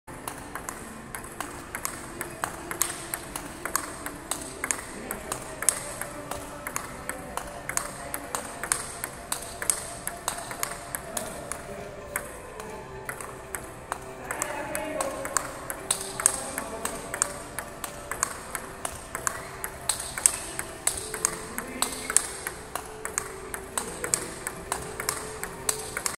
Rajasthan Champion With SAI Coach Mayank Soni Playing FH Topspin against Block
Trisha Verma from Sri Ganganagar, Rajasthan (One of the best Junior, Youth And Womens TTFI Top Athletes) Practicing Forehand Topspin against counter block with Sports Authority Of India (Ministry of youth affairs and sports) Table-Tennis coach Mr. Mayank Soni

Table Tennis Equipments

Trisha Verma (Champion Athlete):
Forehand Black Rubber - Tibhar MXP
Backhand Red Rubber - Tibhar Grass 0.9
Plywood - Yinhe All-rounder

Mayank Soni (SAI Coach) :
Forehand Black Rubber - Butterfly Tenergy 09
Backhand Red Rubber - Butterfly Dignics 07